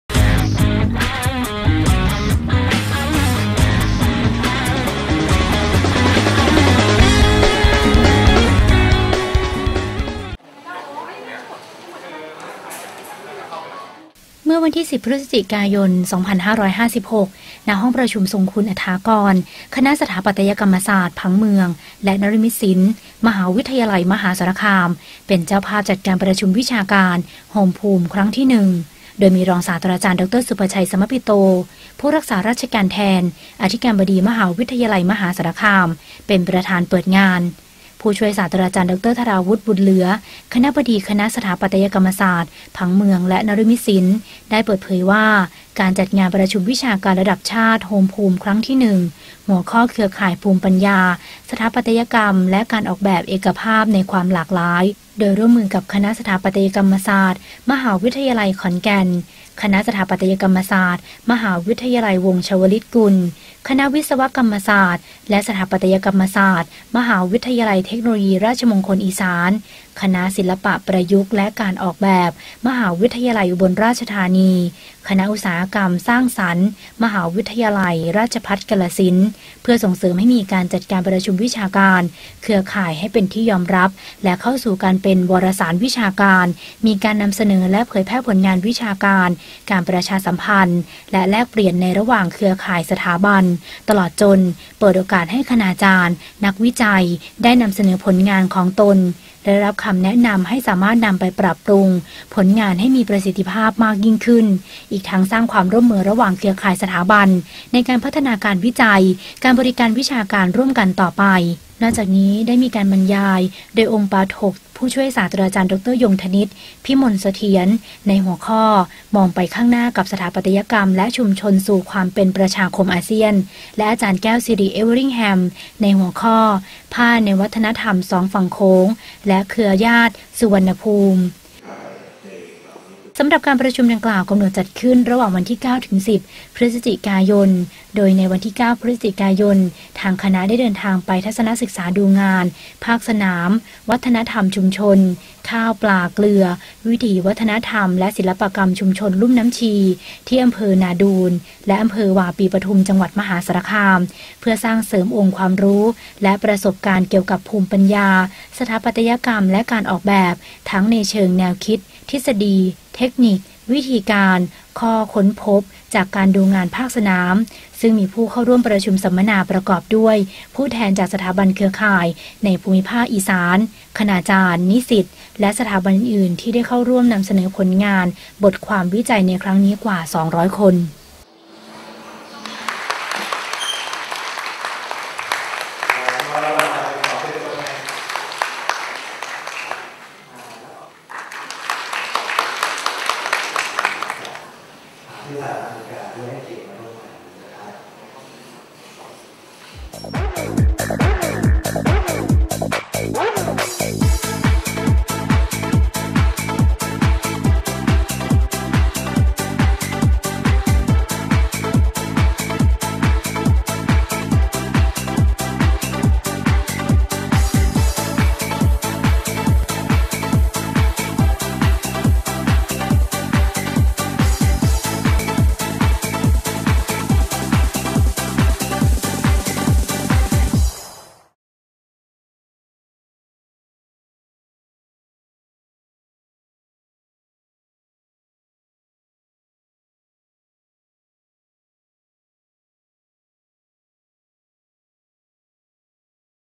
มมส จัดประชุมวิชาการระดับชาติ "โฮมภูมิ" ครั้งที่ 1
เมื่อวันที่ 10 พฤศจิกายน 2556 ณ ห้องประชุมทรงคุณ อัตถากร คณะสถาปัตยกรรมศาสตร์ ผังเมือง และนฤมิตศิลป์ มหาวิทยาลัยมหาสารคาม เป็นเจ้าภาพจัดประชุมวิชาการ "โฮมภูมิ" ครั้งที่ 1 โดยมี รองศาสตราจารย์ ดร.ศุภชัย สมัปปิโต ผู้รักษาราชการแทนอธิการบดีมหาวิทยาลัยมหาสารคาม เป็นประธานเปิดงาน

ภาพ : บุณฑริกา ภูผาหลวง, อภิราม ทามแก้ว และ คณะสถาปัตยกรรมศาสตร์ ผังเมือง และนฤมิตศิลป์
ข่าว : บุณฑริกา ภูผาหลวง
วิดีโอ / ตัดต่อ : พัฒนะ  พิมพ์แน่น
เสียงบรรยาย : ณัฐธิดา  เพ็ชรใต้